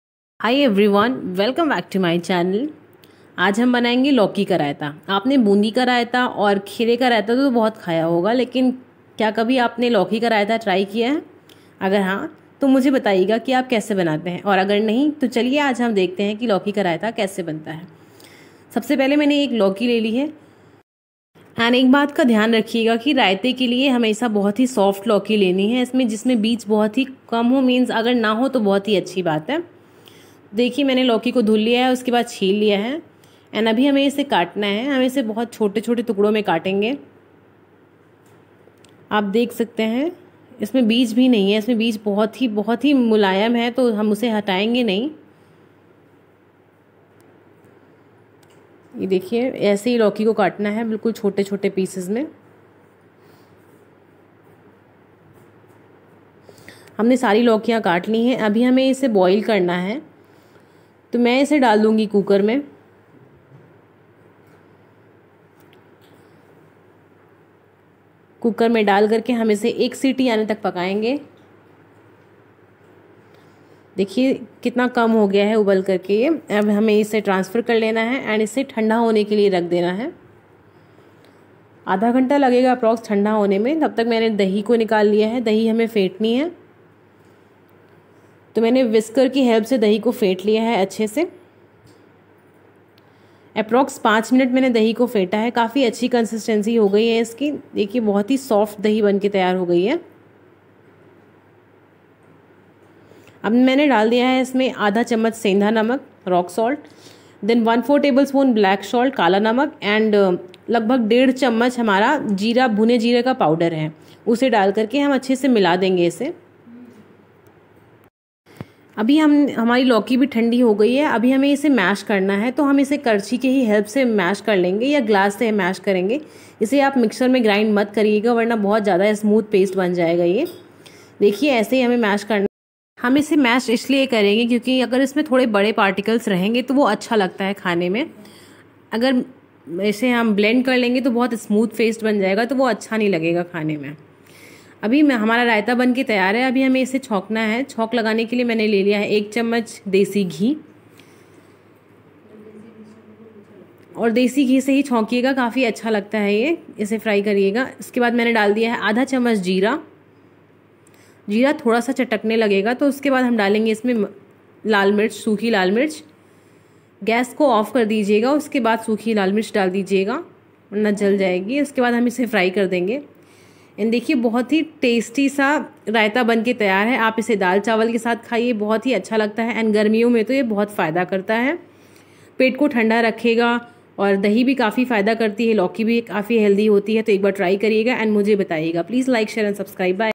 लौकी का रायता| lauki raita recipe
Lauki raita is a very popular recipe for summer it will balance your body temperature and is very tasty you can have it with dal chawal or even only raita as well

Ingredients:
Lauki
Bhuna Jeera powder
kala namak
sendha namak
Dahi

1 bowl lauki raita calories

Do try this raita and let me your experience